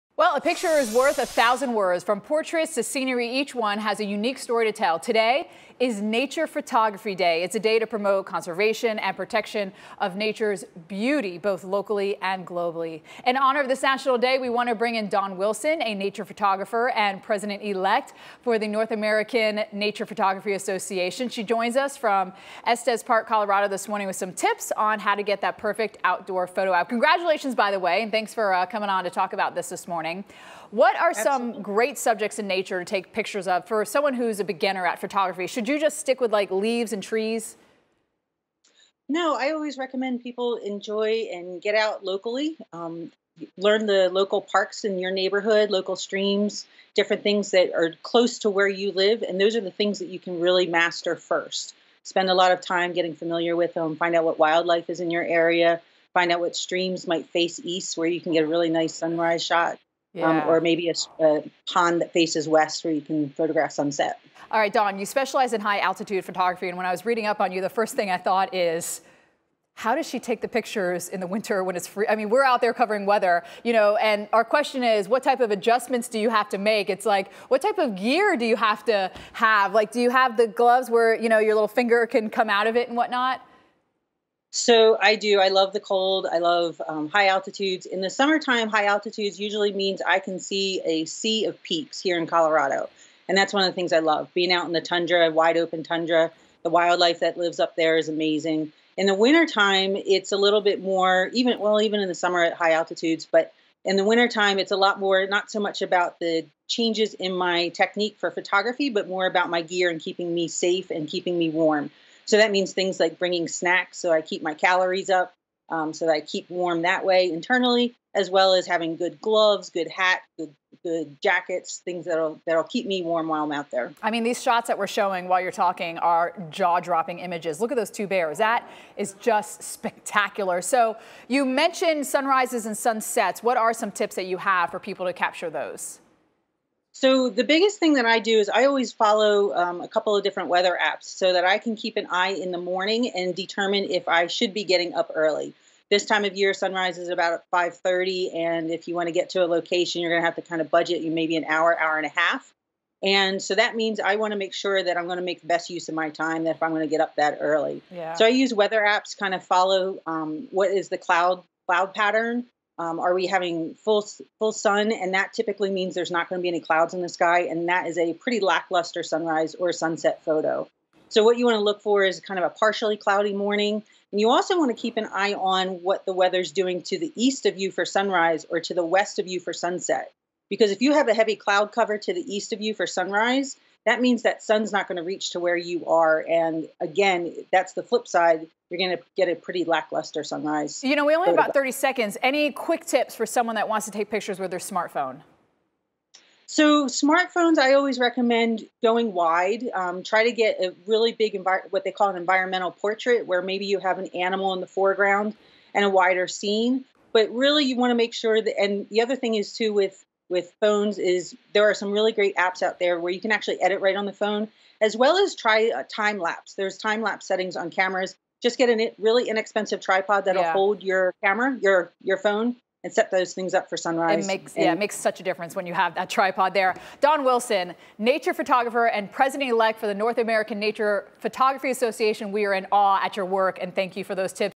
Interview on The Weather Channel: Capturing Nature's Beauty Behind the Lens
On June 15, 2020, I was interviewed by Stephanie Abrams on The Weather Channel to discuss Nature Photography Day and tips for photographing wildlife, landscapes and nature scenes.